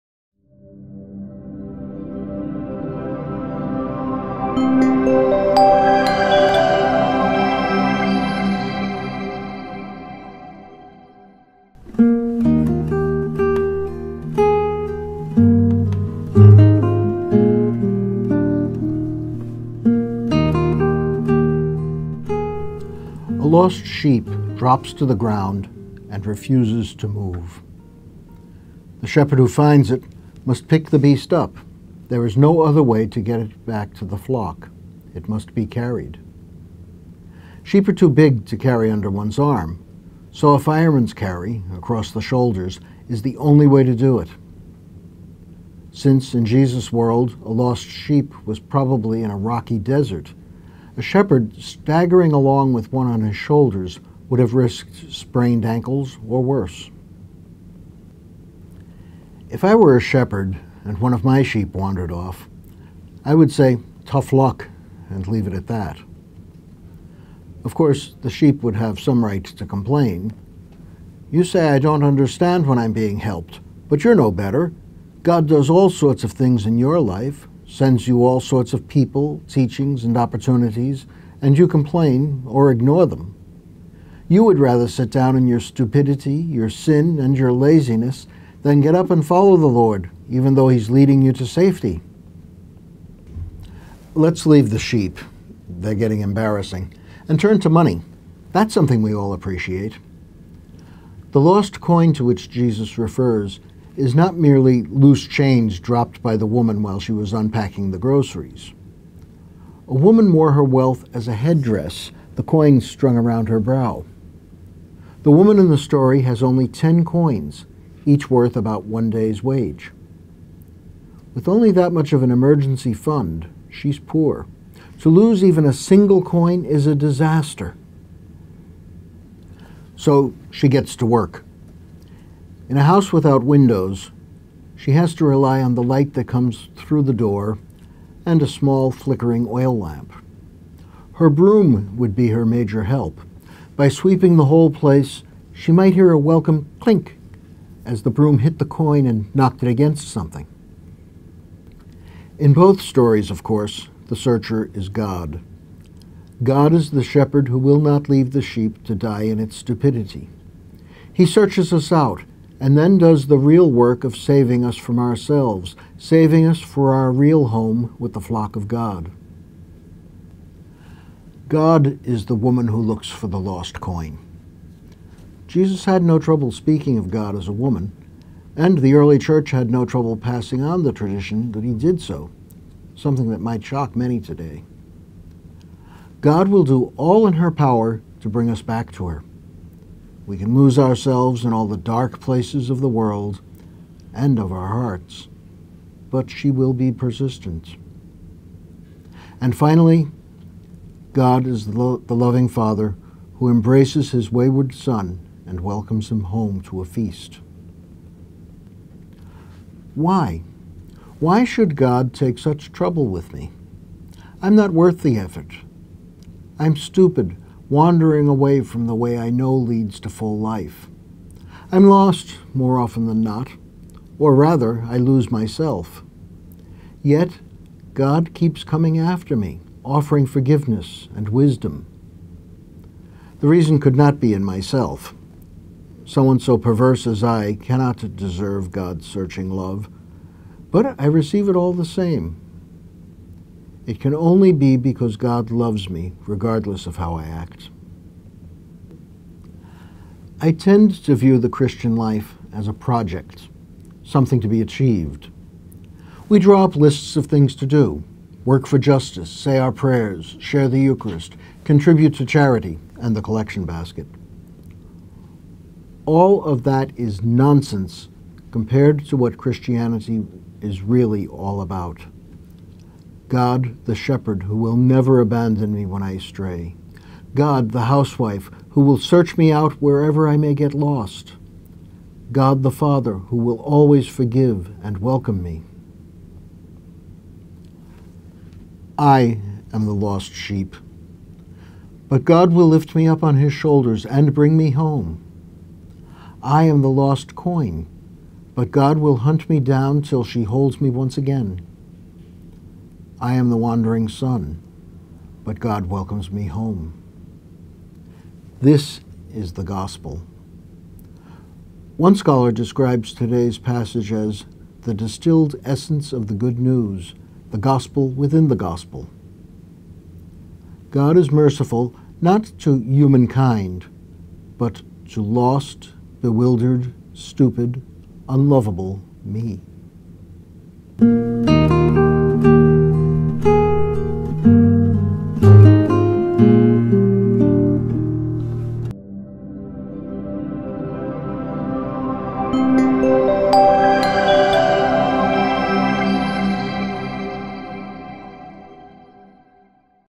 常年期第廿四主日（丙年）
玛利诺会甘伟霖神父主持的主日圣经讲道：有学者形容今天的福音章节是“福音中被提取出的精华，是福音中的福音”，天主是慈悲的，不是泛指对“全人类”，而是对迷路的、困惑的、愚蠢的、不可爱的我